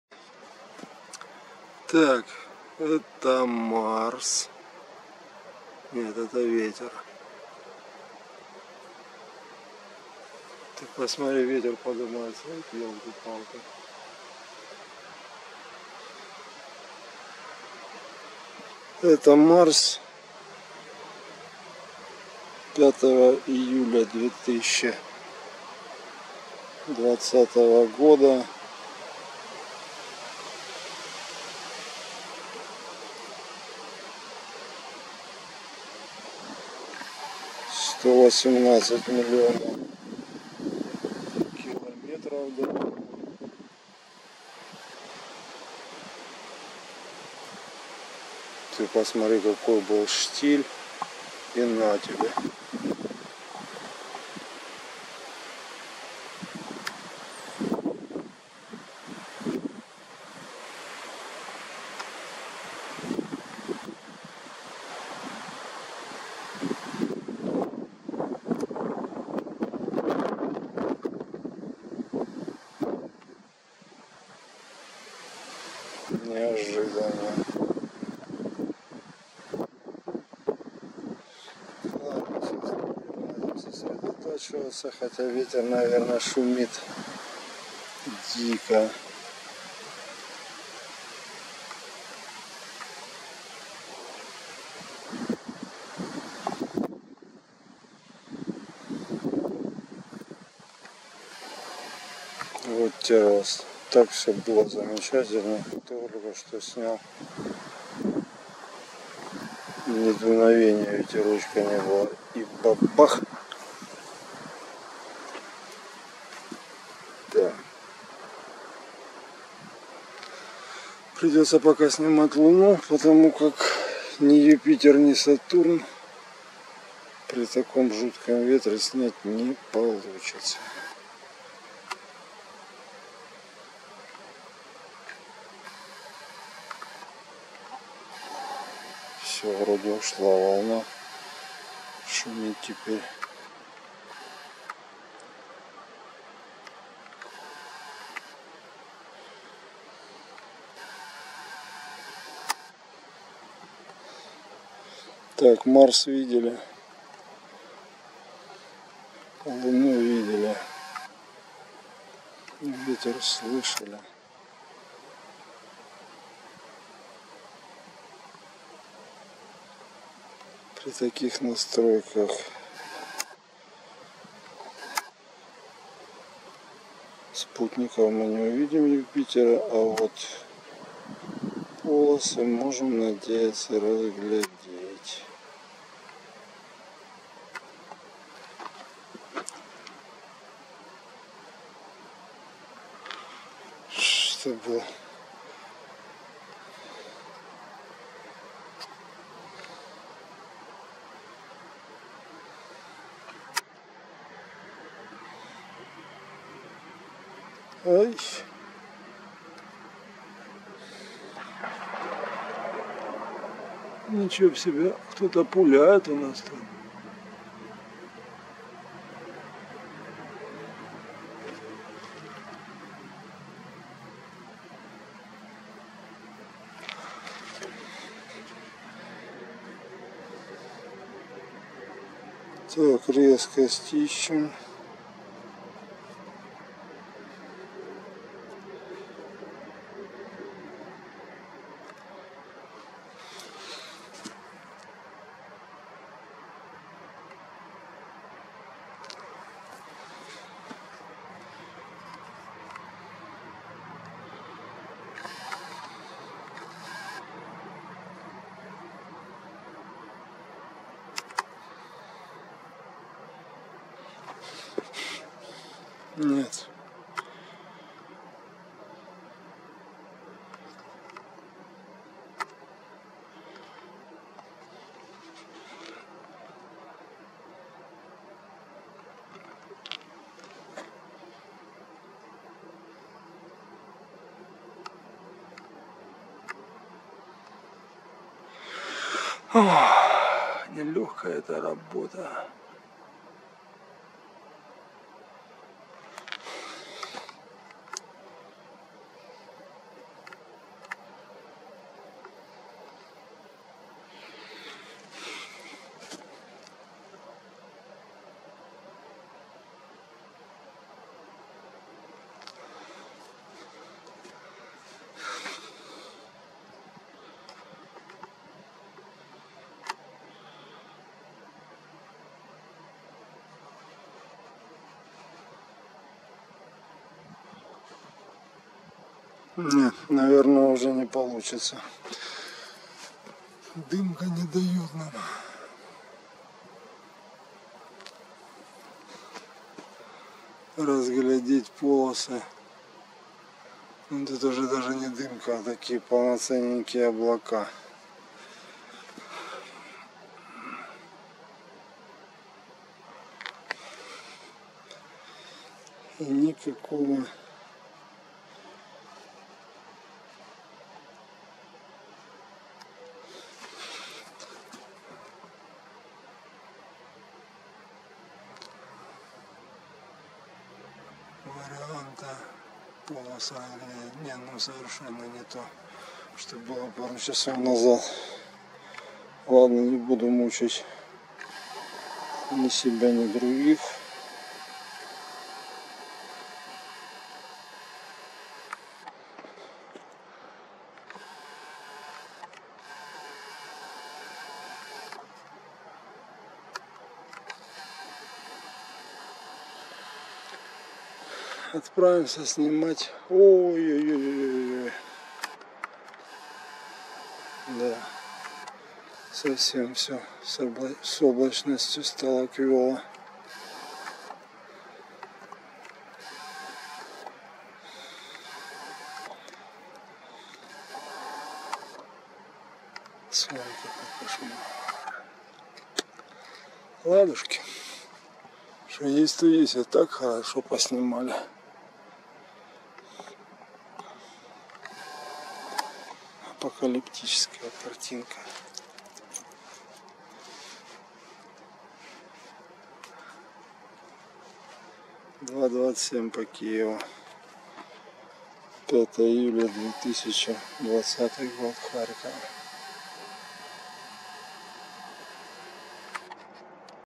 Парад планет 5 июля 2020 Марс, Юпитер и Луна
Марс и Юпитер плюс полная Луна на параде планет 5 июля 2020 года